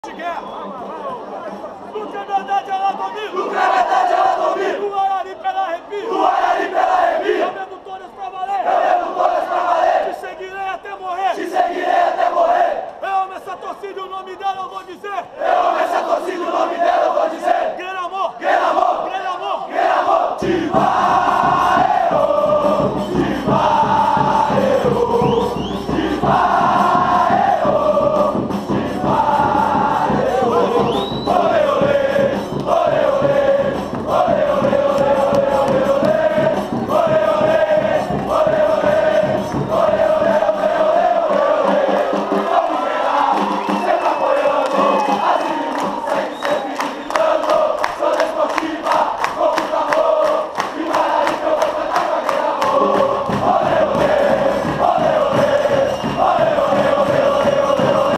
GRENAMOR - Grito de guerra / Entrada do time
TOG no Engenheiro Araripe para o jogo Desportiva x Doze pelo Capixabão 2016 (13/02/2016).

No Kleber Andrade ela domina
No Araripe ela arrepia
Eu bebo todas pra valer
Te seguirei até morrer
Eu amo essa torcida e o nome dela eu vou dizer
Grenamor
Grenamor
Tiva êo